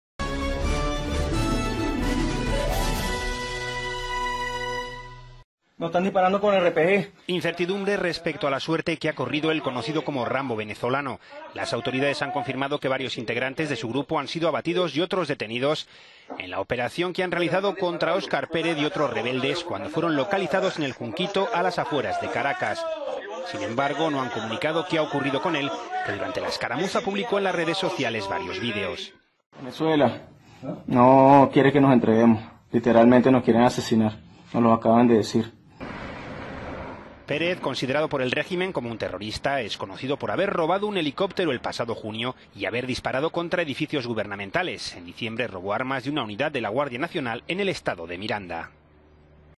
Así Mataron a Óscar Pérez expolicía venezolano
Un operativo para capturar al expolicía venezolano Óscar Pérez, quien en 2017 atacó desde un helicóptero edificios del gobierno, dejó el lunes dos policías y un número no precisado de “terroristas” muertos, según el gobierno.